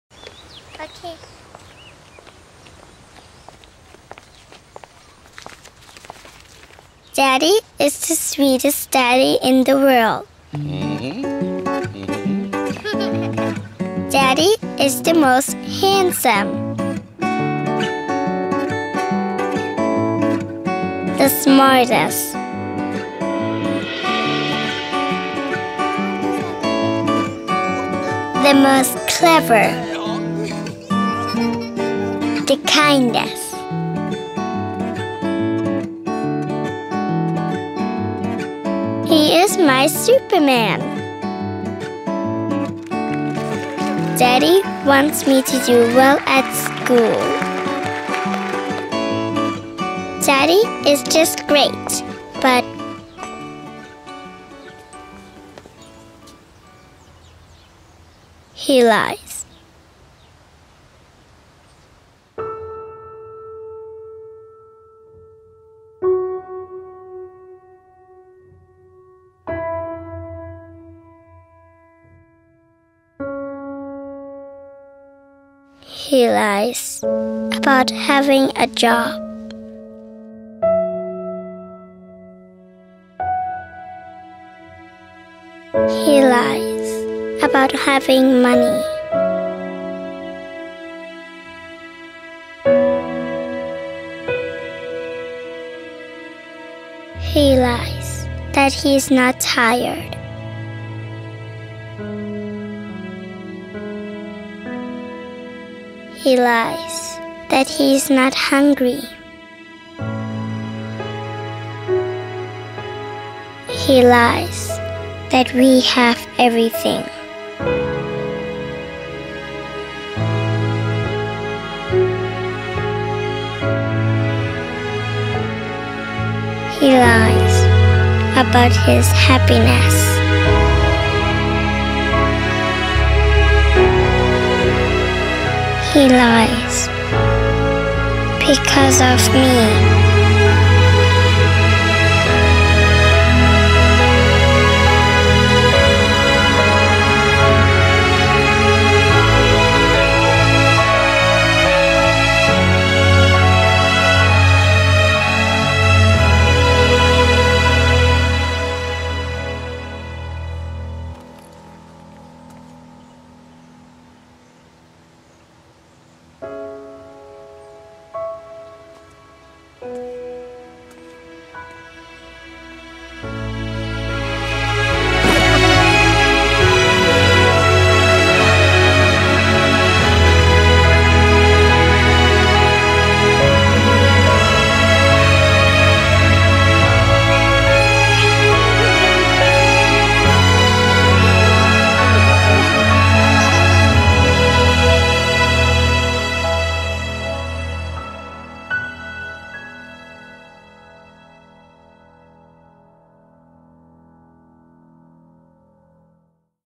Kebohongan Seorang Ayah ( Jutaan Orang Menangis Karena Video Ini )
Ayah Pembohong Ini Bisa Membuat Anda Menangis

BANYAK iklan komersial yang sengaja dibuat untuk menggerus perasaan. Menguras air mata. Iklan-iklan asuransi yang tayang di stasiun-stasiun televisi Thailand, misalnya. Dengan pendekatan seperti ini, iklan-iklan tersebut berhasil mencapai tujuannya, yakni melekat di benak pemirsa. Bahkan saat disebarluaskan lewat laman sosial, termasuk youtube, penontonnya berderet-deret hingga jutaan orang.

Awal tahun 2015, satu iklan sejenis tayang di sejumlah stasiun televisi di Hongkong. Iklan yang menggambarkan hubungan antara ayah dan anak. Ayah yang hendak membahagiakan anaknya, dan anak yang pada akhirnya memang berbahagia, meski mengetahui betapa sesungguhnya, kebahagiaan ayahnya dibangun atas upaya yang sungguh-sungguh bertolak belakang dari kebahagiaan itu sendiri.

Sang ayah bersikap seolah-olah tidak ada masalah apapun. Bersikap seolah-olah ia seperti ayah-ayah teman anaknya yang serba berkecukupan. Padahal ia sebenarnya tengah lintang-pukang mencari pekerjaan dan kemudian menerima pekerjaan apa saja, termasuk membersihkan kaca dan menjadi kuli panggul, untuk mendapatkan uang.

Tapi di hadapan anaknya, seorang gadis kecil, ia selalu berusaha tampak keren. Ia mengenakan jas dan dasi setiap hari, dan baru membukanya di tempat kerja. Ia mengira anaknya tidak tahu. Padahal anaknya tahu. Anaknya tahu dia berbohong.

He is lie about having a job.
He lies about having money
He lies that he's not tired
He lies that is not hungry
He lies that we have everything
He lies about his happiness

Namun kebohongan ini, meski menohok, disadari oleh si anak, dilakukan sang ayah untuk kebahagiaannya. "He lies because of me..."medan,tribunnews,com